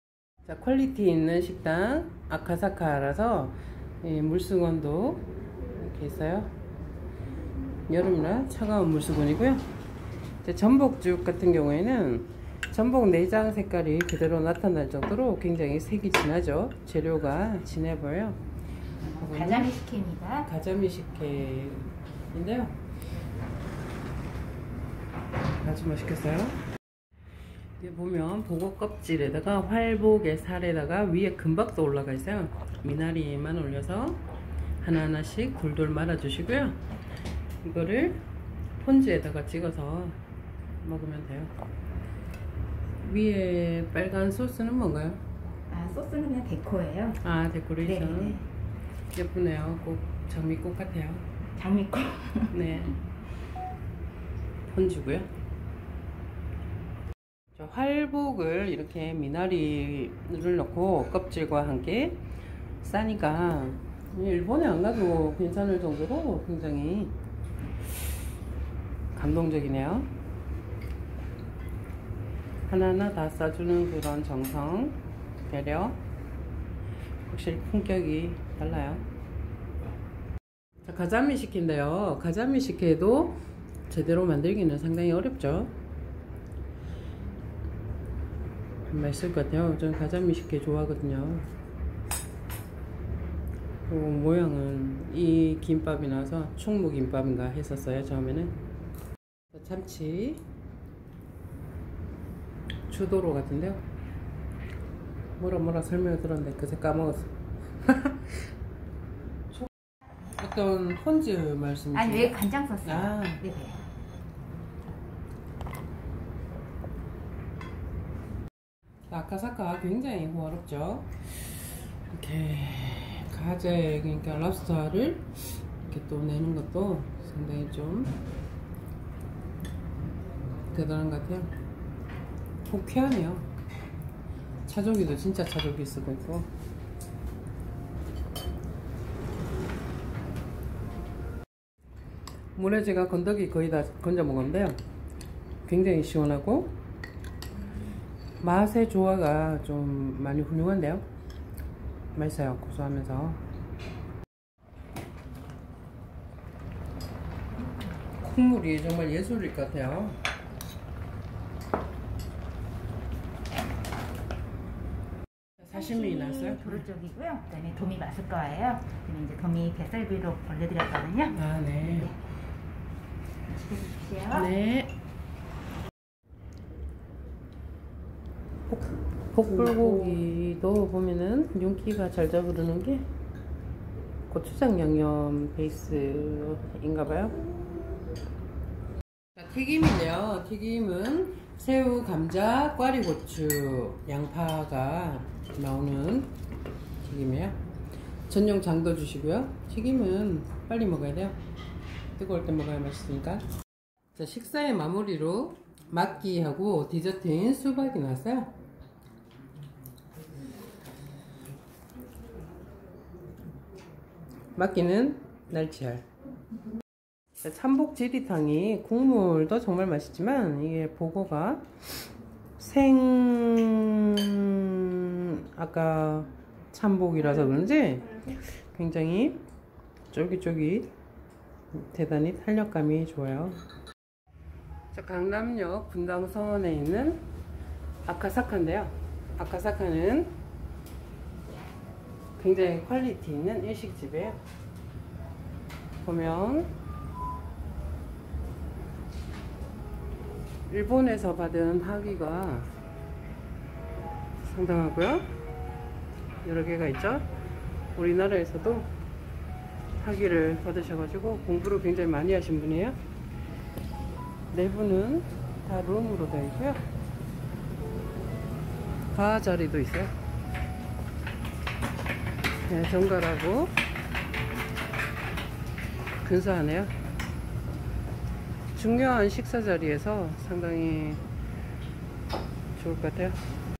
강남역 고급 일식당 2인 30만원 아카사카 활복코스
강남역 아카사카의 1인 15만원, 2인 30 만원짜리 호화로운 생참복 코스요리 먹고 왔어요.